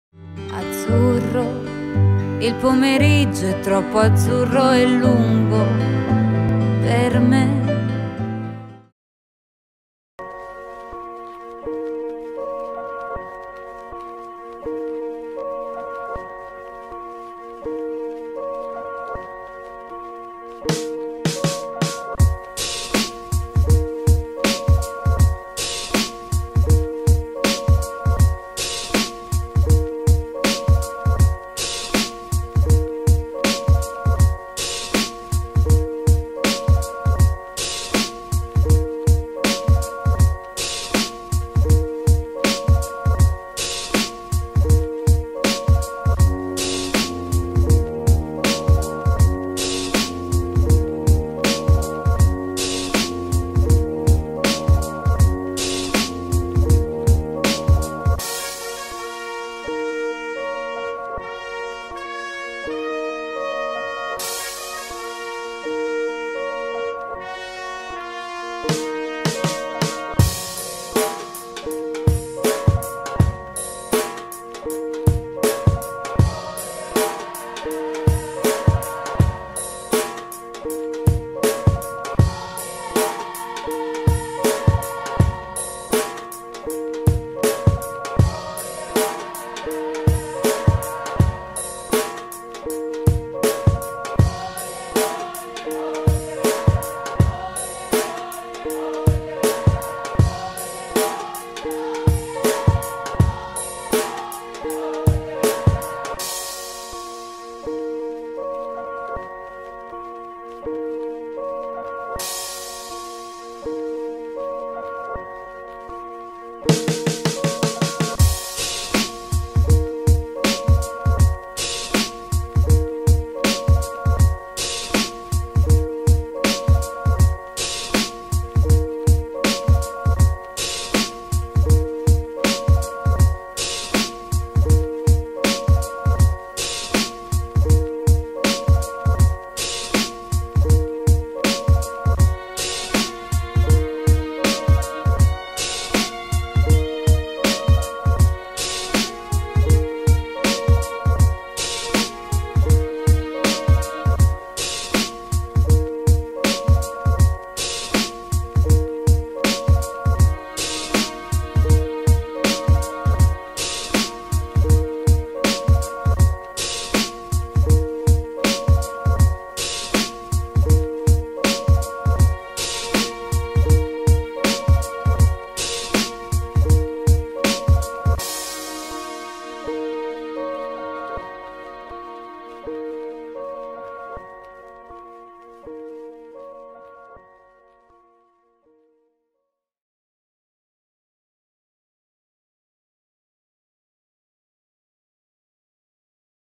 VIDEO NATURAL TREKKING - IISS Cosmai - Trani
Escursione naturalistica per il progetto PTOF,  presso la riserva di Torre Guaceto con gli alunni del corso "audiovisivo" e "moda" dell'IISS Cosmai di Trani.